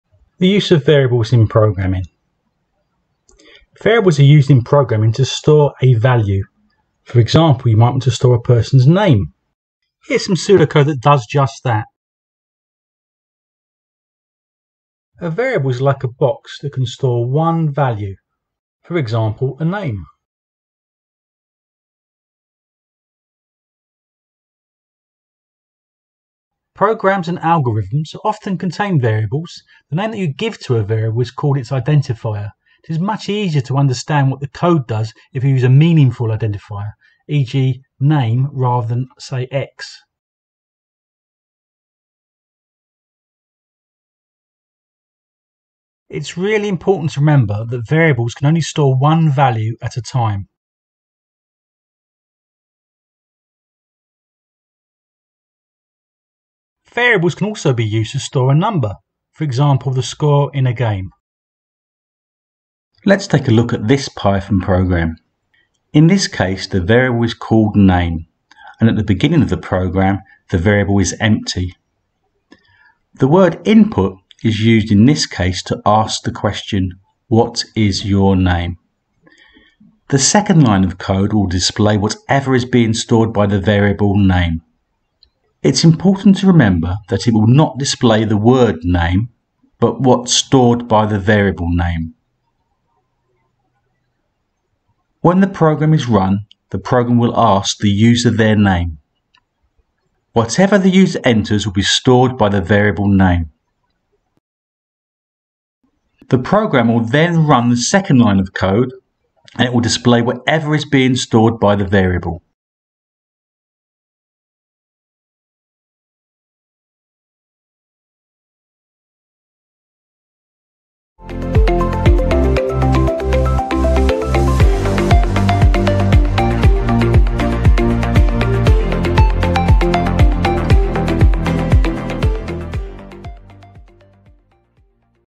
Computing Basics - The use of variables in programming
This video provides an introduction to the use of variables in programming.
It's best if you watch this video before the video that explains data types.
Python is used as the programming language in this video.